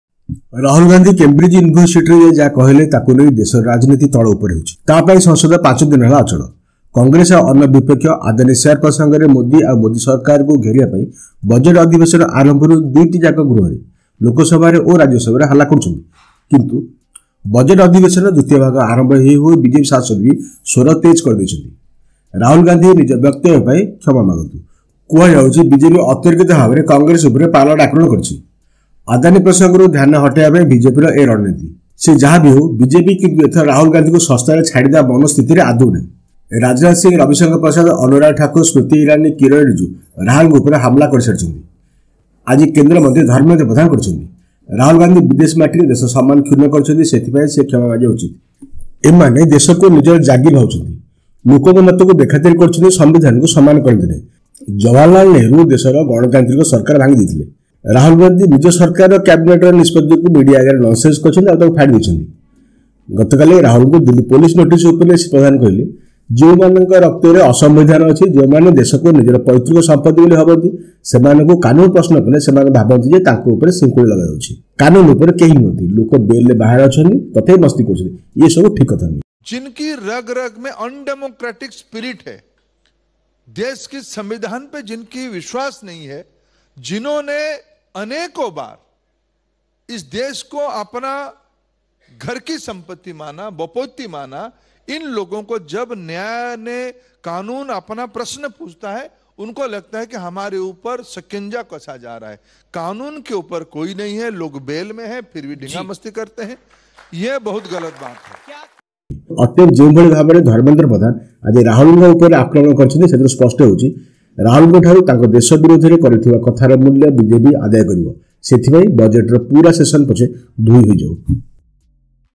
Rahul must apologize, no one is above the law Dharmendra Pradhan slams
Rahul must apologize, no one is above the law Dharmendra Pradhan slams dharmendrapradhan rahulgandhi apology